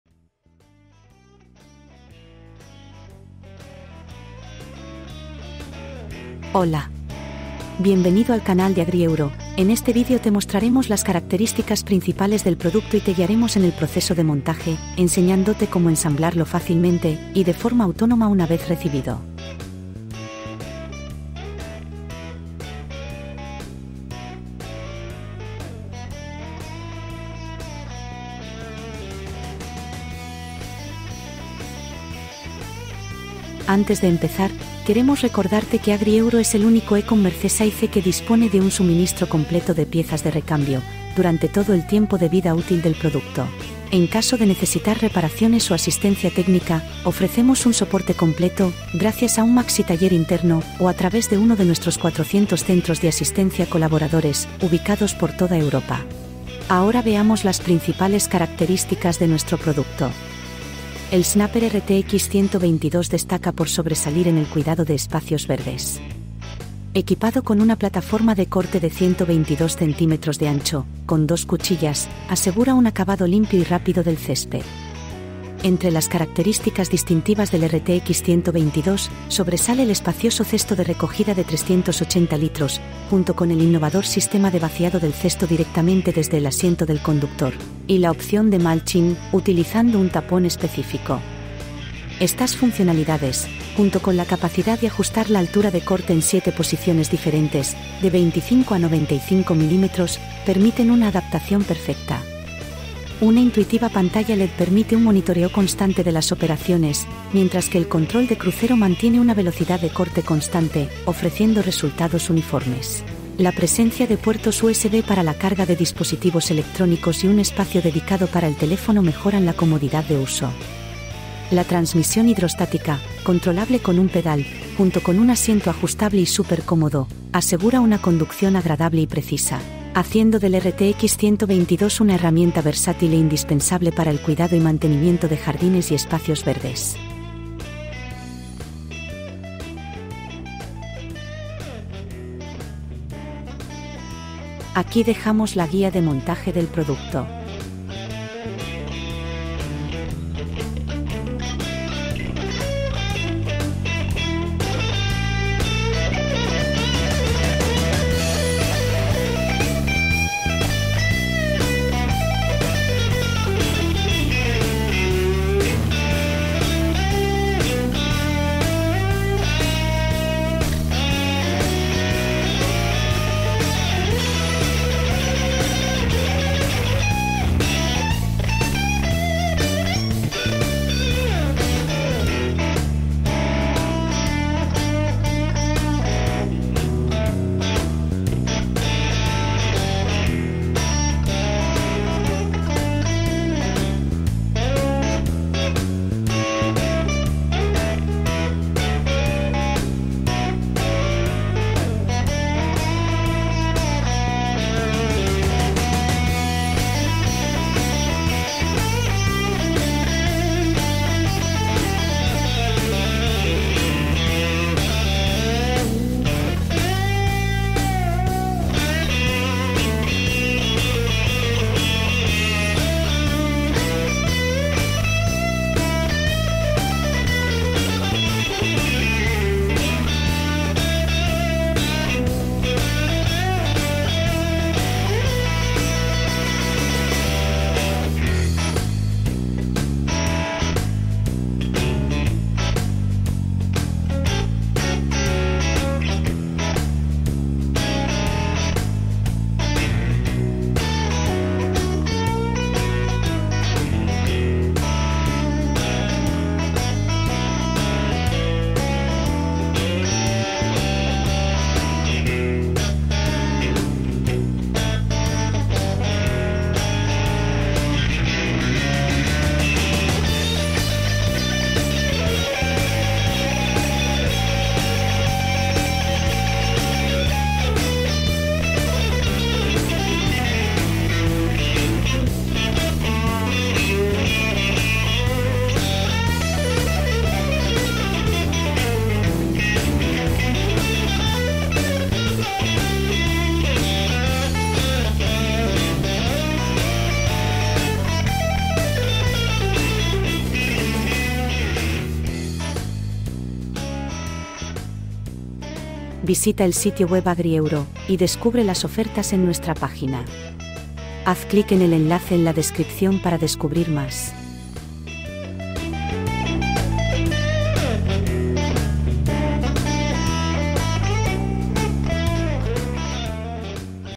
Presentación y montaje: Snapper RTX122 - Tractor cortacésped - Briggs&Stratton 724cc - Recogedor
*Aquí tiene un vídeo de presentación y montaje del Snapper RTX122 - Tractor cortacésped - Briggs&Stratton 724cc - Recogedor - Mulching, realizado directamente por nosotros en AgriEuro para mostrarle todas las características principales.*

0:00-0:05 Introducción
0:06-0:34 Desembalaje
0:34-0:56 Pedir recambios
1:01-2:23 La máquina
2:28-5:00 Guía de montaje
5:01-5:05 360° del producto
5:06-5:13 Info del sitio
5:16-5:24 Fin

El tractor cortacésped Snapper RTX122 es una opción de máxima eficiencia con una calidad superior. Diseñado para aquellos encargados del cuidado de extensas áreas verdes, combina tecnología avanzada con facilidad de uso.

Equipado con un potente motor Briggs & Stratton 8270 PXi V-Twin OHV de 724 cc, el Snapper RTX122 ofrece un rendimiento fiable y potente. Su aspecto moderno y características innovadoras, como una amplia plataforma de corte de 122 cm con dos cuchillas, lo convierten en la elección perfecta para un corte limpio y rápido.

Ofrece dos opciones de corte:

- Un gran recogedor de 380 litros, que maximiza la eficiencia del trabajo al reducir el tiempo de vaciado. La practicidad se incrementa con la posibilidad de vaciar el recogedor directamente desde el asiento del conductor mediante una cómoda palanca.
- Corte mulching gracias al tapón suministrado, que se introduce en el túnel trasero.

Entre las características especiales del RTX122 se incluyen:

- Una pantalla LED que facilita el seguimiento de las operaciones del tractor.
- Un control de velocidad cruise para mantener siempre la misma velocidad de corte.
- Puertos USB para cargar dispositivos electrónicos y un compartimento específico para guardar el teléfono.
- Un asiento súper cómodo que se puede ajustar para mayor comodidad durante el trabajo.

El Snapper RTX122 también cuenta con corte en marcha atrás, facilitando la manipulación en espacios complejos.

Con capacidad para ajustar la plataforma de corte en 7 posiciones diferentes, de 25 a 95 mm, garantiza un resultado impecable en diversos tipos de superficies. La transmisión hidrostática controlada con un pedal proporciona una experiencia de conducción cómoda, segura y precisa, tanto hacia delante como hacia atrás.

Equipado con un enganche de remolque, el Snapper RTX122 es una herramienta versátil que permite el uso de accesorios adicionales para el cuidado del jardín, convirtiéndolo en una opción eficiente y multifuncional para todas las necesidades de mantenimiento verde.